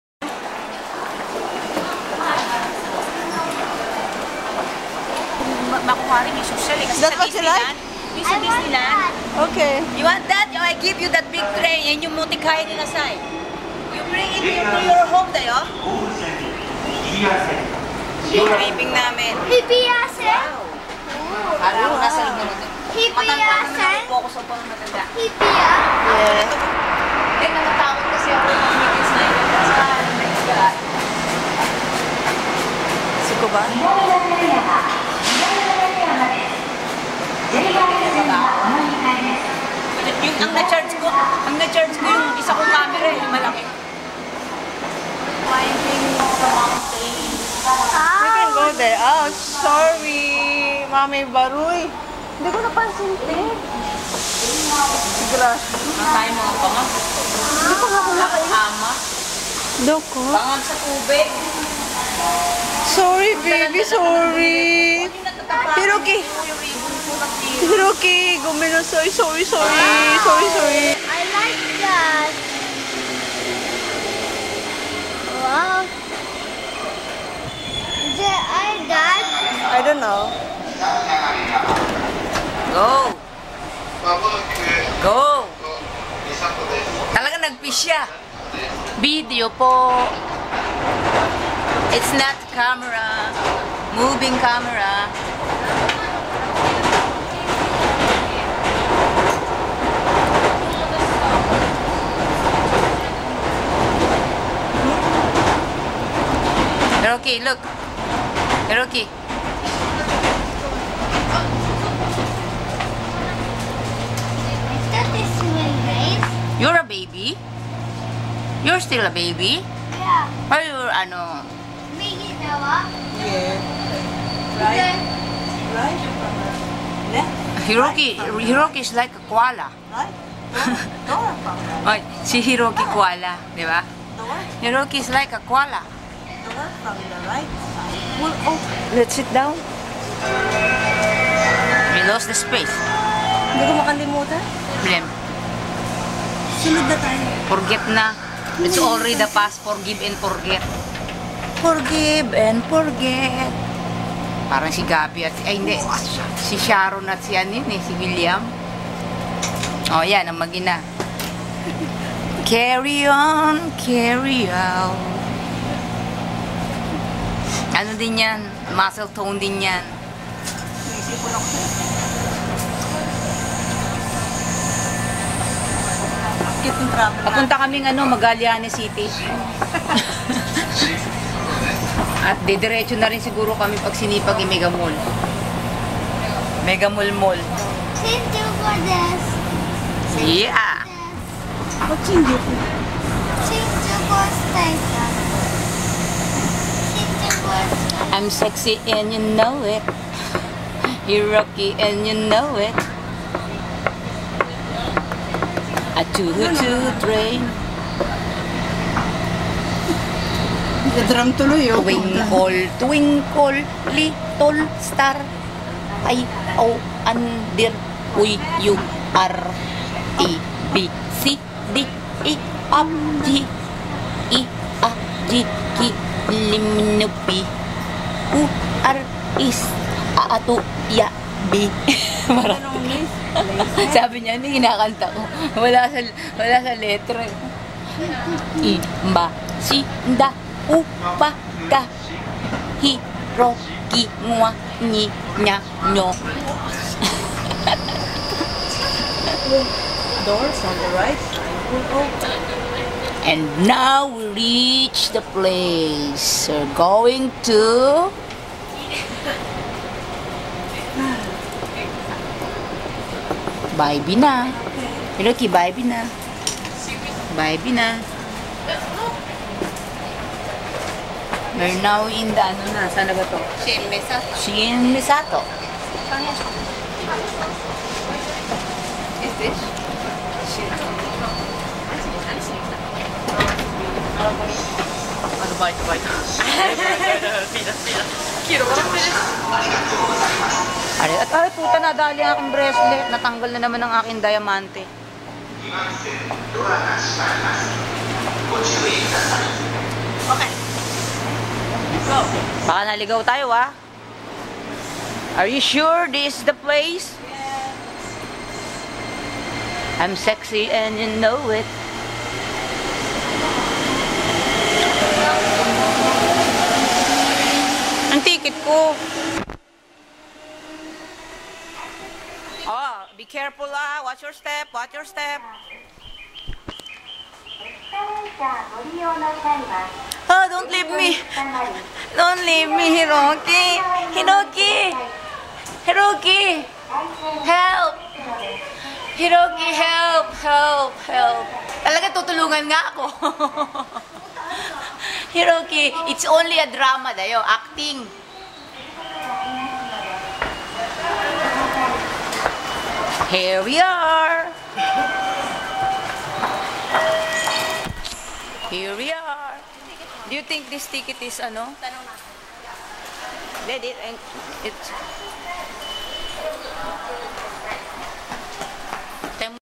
On our way to Thomas Land
See u later... Thomas Hiroki is coming. , We need to travel by train for about 30 to 40 minutes to go to Shim Misato, It's a place where Thomas town is located, obviously I love the train very much...